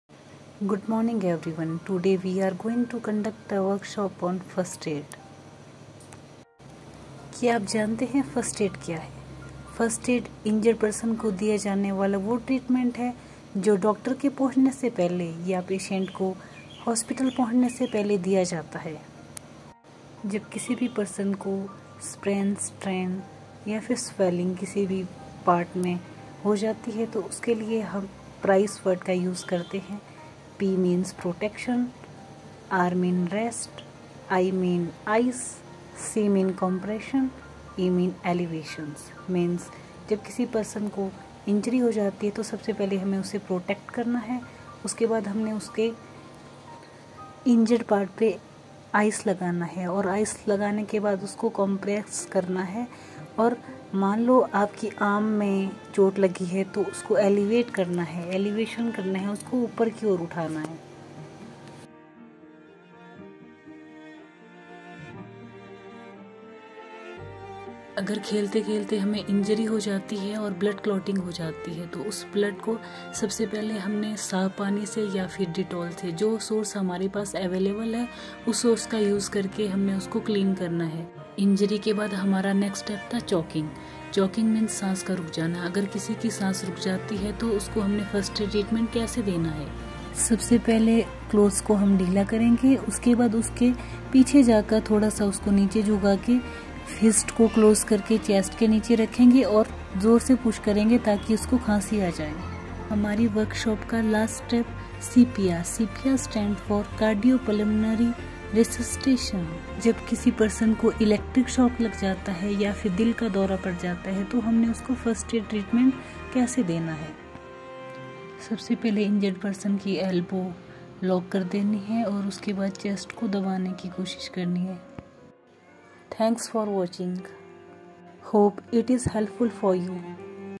Highlander's First Aid workshop🏥
First aid refers to medical attention that is usually administered immediately after the injury occurs and at the location where it occurred.
The recommended treatment for most acute injuries is referred to as the PRICE principle. This is an acronym for protection, rest, ice, compression, and elevation.
First aid for a choking person If a person is choking and cannot talk, cry or laugh forcefully, give five back blows and then five abdominal thrusts.
CPR-cardiopulmonary resuscitation
Giving CPR
Hand position: Two hands centered on the chest.
Body position: Shoulders directly over hands; elbows locked.
Depth: At least 2 inches.
Rate: 100 to 120 per minute.
Allow chest to return to normal position after each compression.